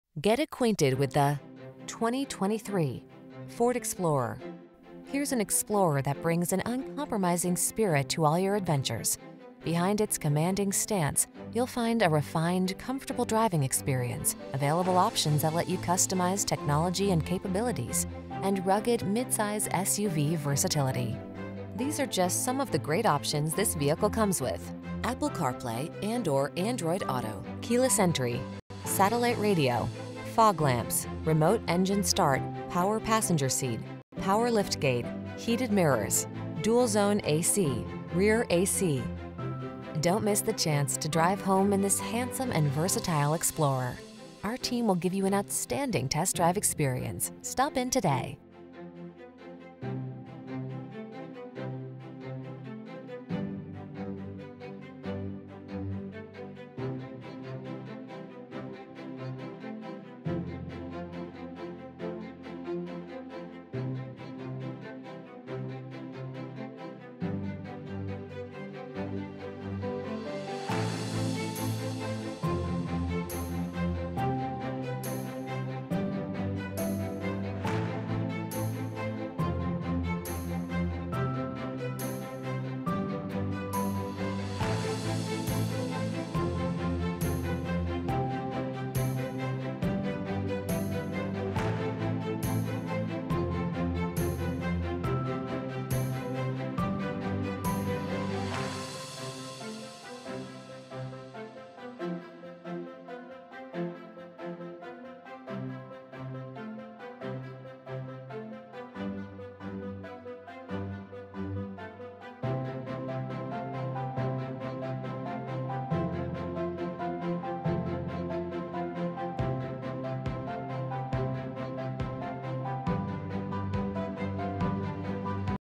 2023 Ford Explorer Charlevoix, Petoskey, Gaylord, Traverse City, Cheboygan C23X005
Blue Metallic New 2023 Ford Explorer XLT available in 6725 US Highway 31 South, Charlevoix, Michigan at Jim Riehls Friendly Ford. Servicing the Charlevoix, Petoskey, Gaylord, Traverse City, Cheboygan, area.

2023 Ford Explorer XLT - Stock#: C23X005

2023 Ford Explorer XLT Stone Blue Metallic 10-Speed Automatic 2.3L EcoBoost I-4 4WDbrbr4WD. 20/27 City/Highway MPG